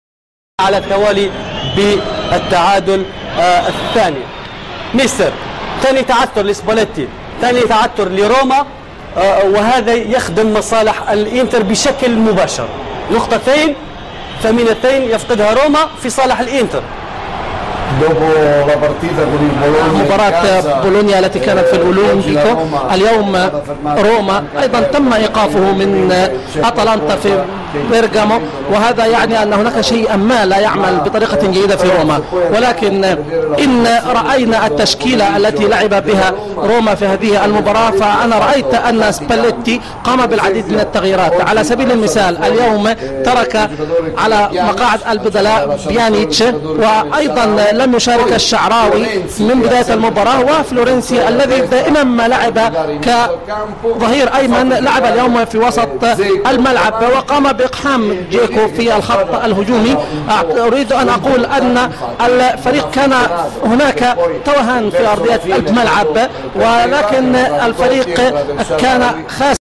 اهداف مباراة أتلانتا و روما 3-3 || الاهداف كاملة || الدورى الايطالى ||17-4-2016
اهداف مباراة روما  وأتلانتا 3-3 الاحد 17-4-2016 الدورى الايطالى ||